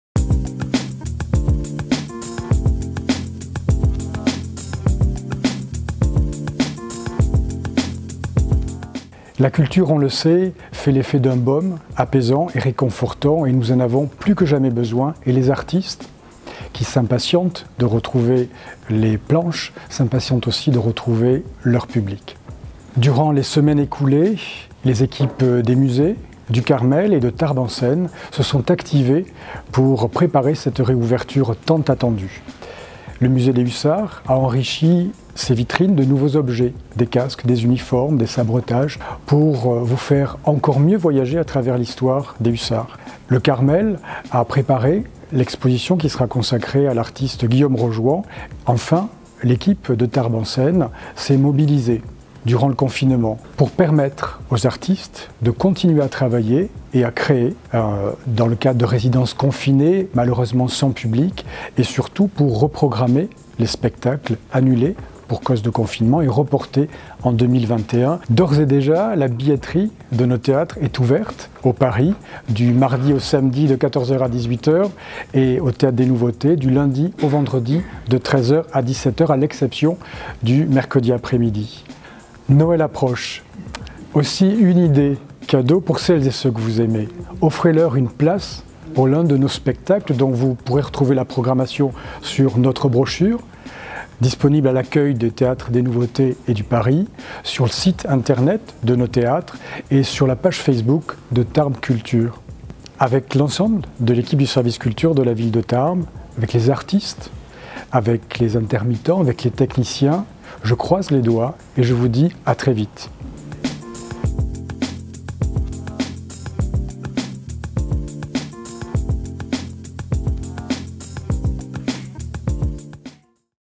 La culture à Tarbes
Philippe Lasterle, Adjoint au maire en charge de la culture, fait un point sur la continuité de l'action culturelle à Tarbes  et dans les salles de spectacles.